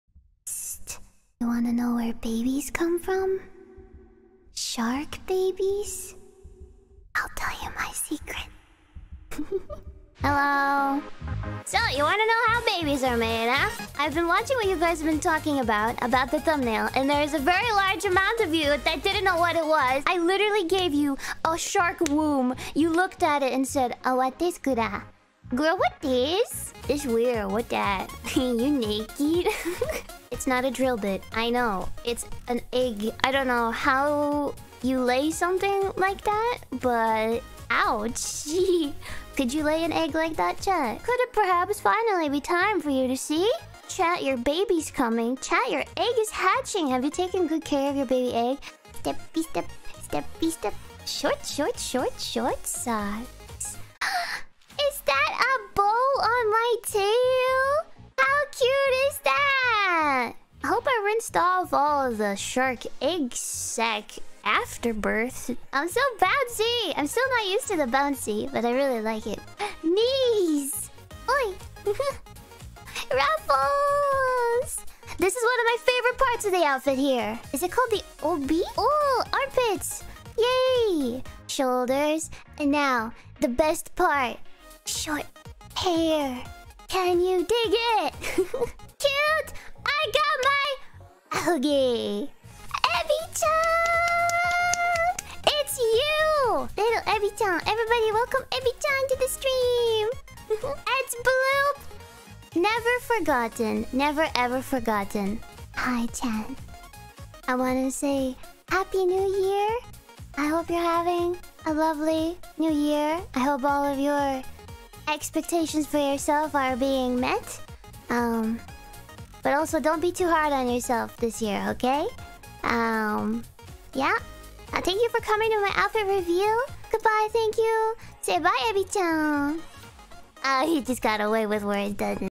Gura's Kimono Outfit reveal started with Baby Making Facts - SHE IS ADORABLE !!
Gura's outfit reveal ! She is adorable
Stream was cute and a bit yabai as always !

Timestamps:
00:00 Gura whispers
00:15 Gura talks about shark womb
00:25 You nakey Gura ?
00:35 Egg
01:00 Legs Reveal
01:15 Cute Obi
01:25 Gura got ahoge
01:35 Shimp on Gura's head, She drew it herself !
01:46 Gura new years wishes for Chat

Here's my twitter if you wanna follow